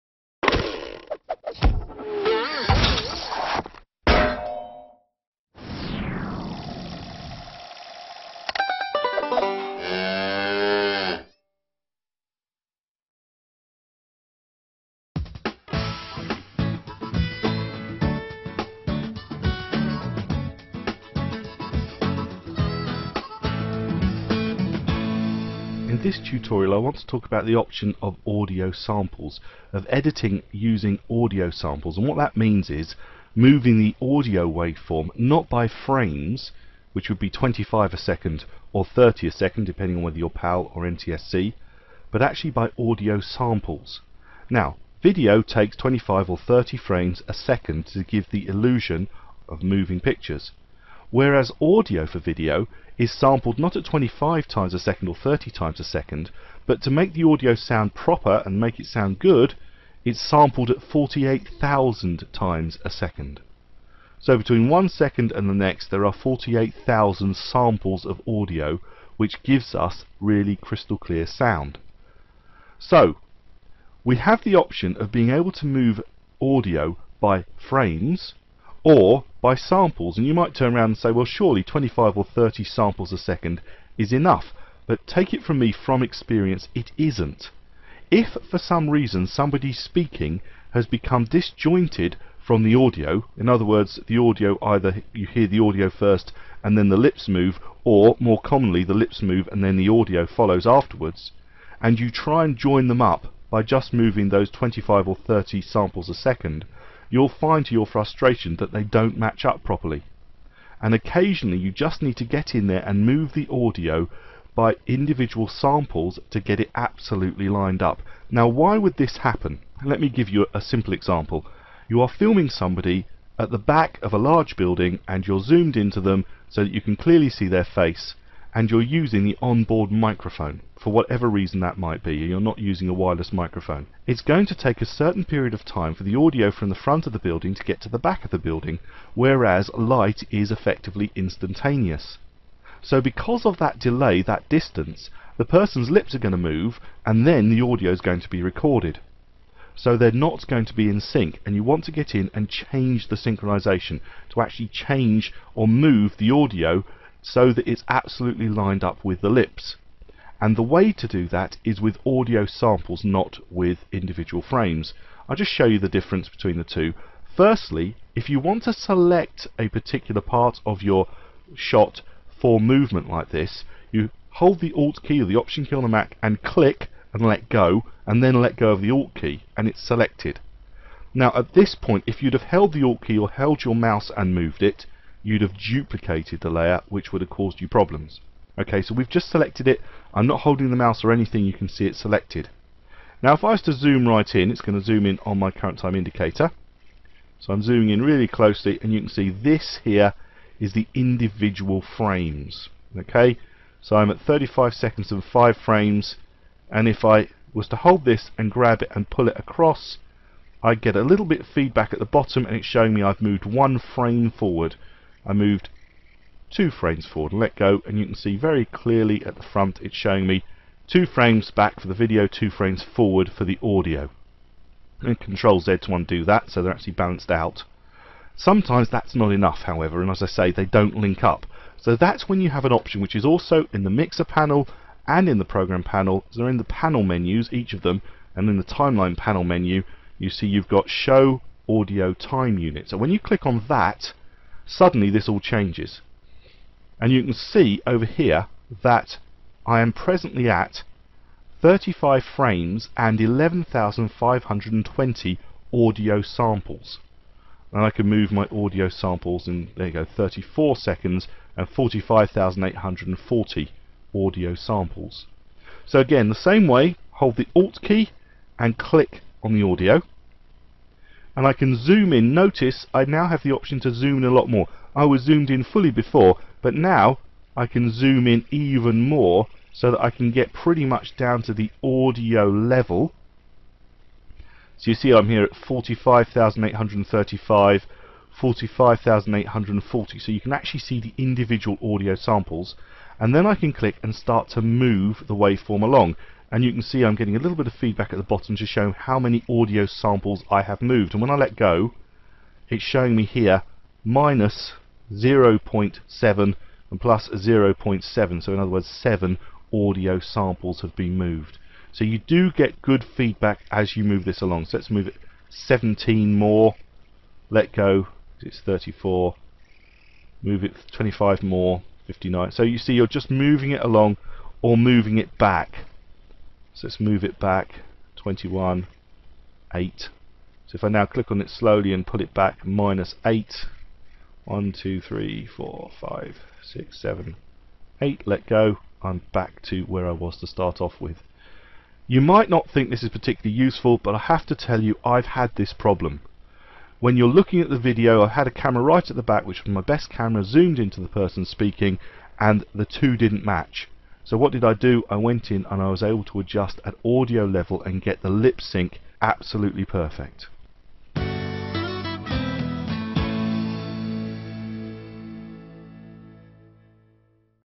Premiere Pro CS6 Techniques: 84 Audio 14: Audio Samples
In this tutorial, Andrew Devis talks about the difference between frames and audio samples in video and shows how you can work with audio samples to deal with things like lip-sync problems with video shot at some distance from the original speaker.